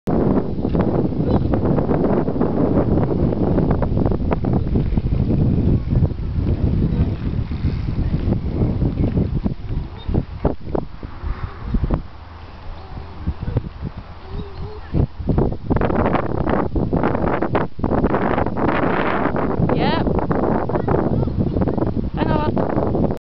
Jonathon on his Monty part 2
Jonathon aged 2 on his Monty at 'the track' in cornwall. Stabilisers not required! or brakes even.